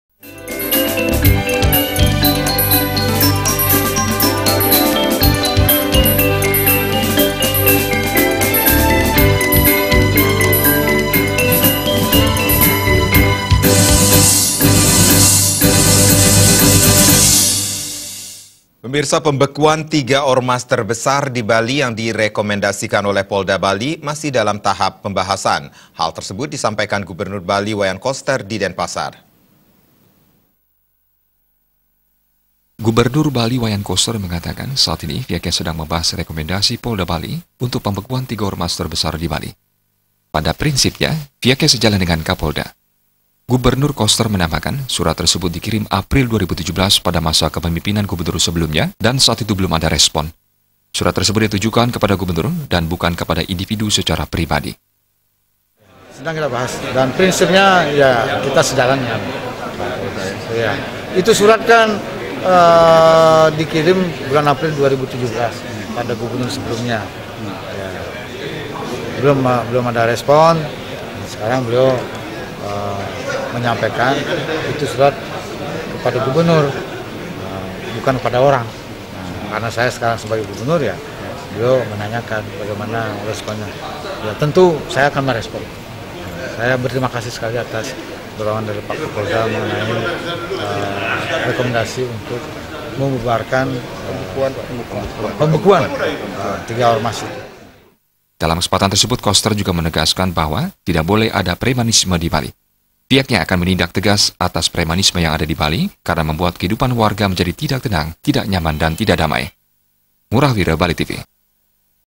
TINDAK LANJUTI REKOMENDASI TERKAIT PEMBEKUAN TIGA ORMAS
PEMBEKUAN TIGA ORMAS TERBESAR DI BALI YANG DIREKOMENDASIKAN OLEH POLDA BALI, MASIH DALAM TAHAP PEMBAHASAN. HAL TERSEBUT DISAMPAIKAN GUBERNUR BALI, WAYAN KOSTER DI DENPASAR.